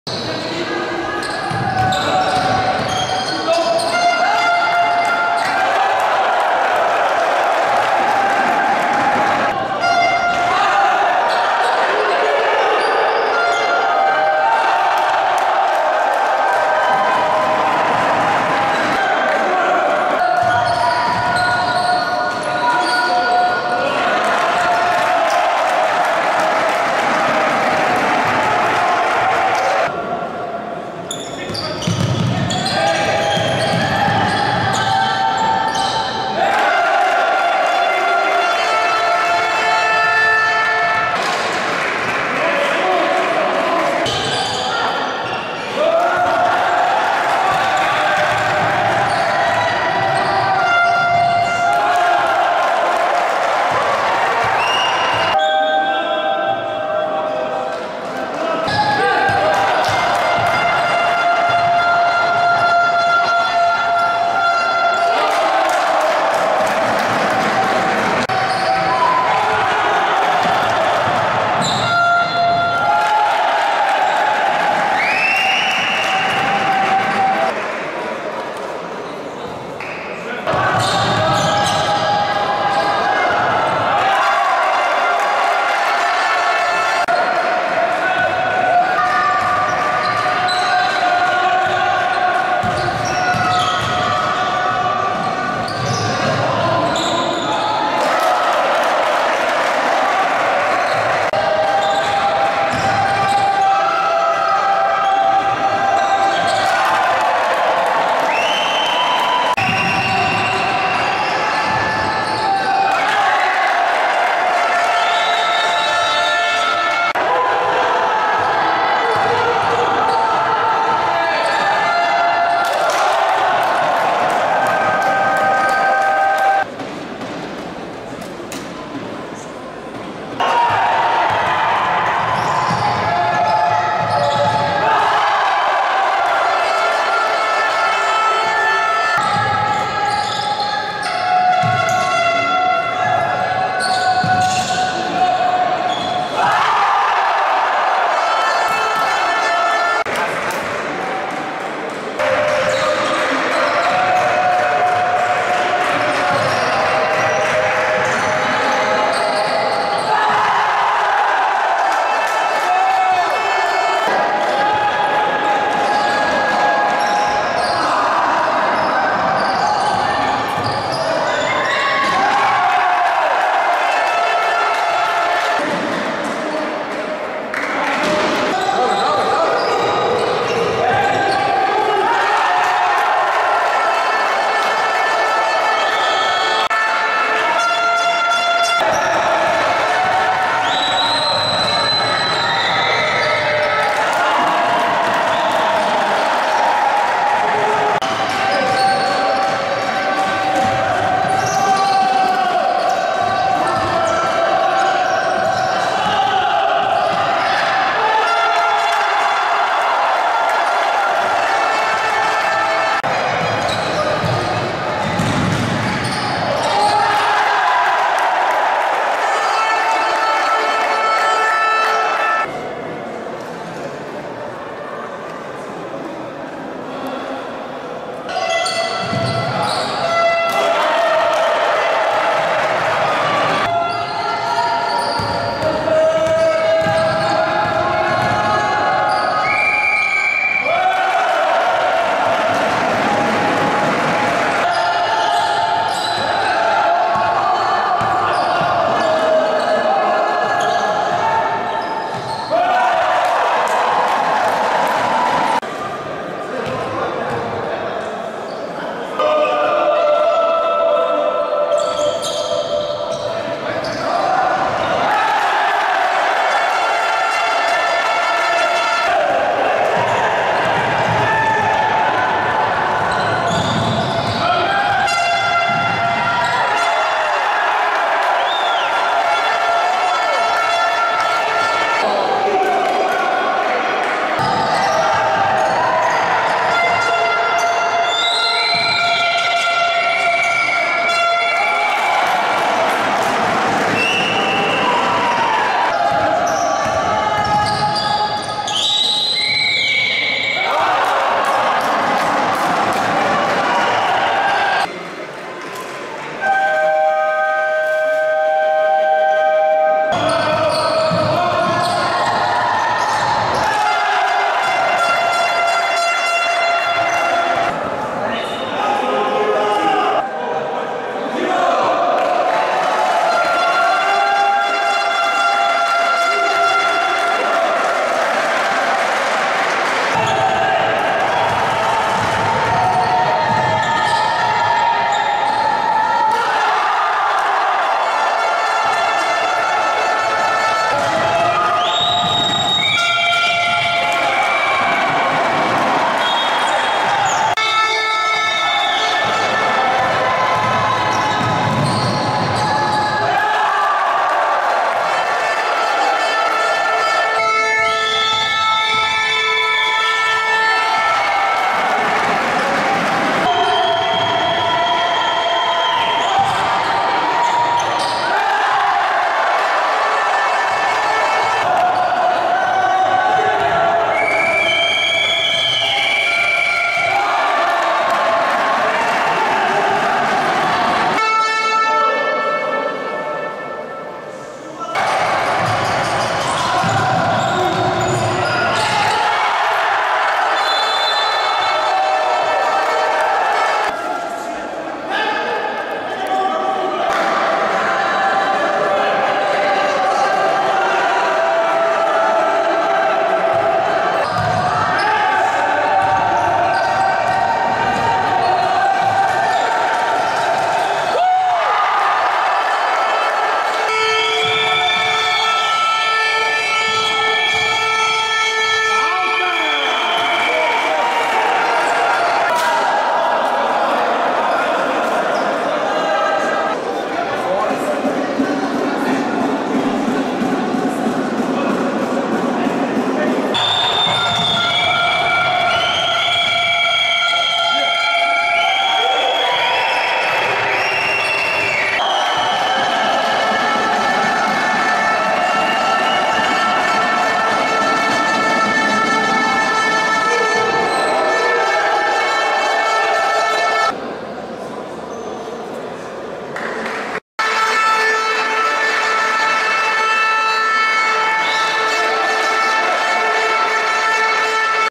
Basquetebol - Resumo da Final: GDB Leça - UAA Aroso U18M MAI2023
Basquetebol - Final Torneio Interassociações Sub 18 Masculinos: GDB Leça, 57 - UAA Aroso, 51. Resumo do jogo realizado no Pavilhão do GRIB - Grupo Recreativo Independente Brandoense, em Paços de Brandão, na tarde de domingo, dia 21 de maio de 2023. O GDB Leça venceu a competição.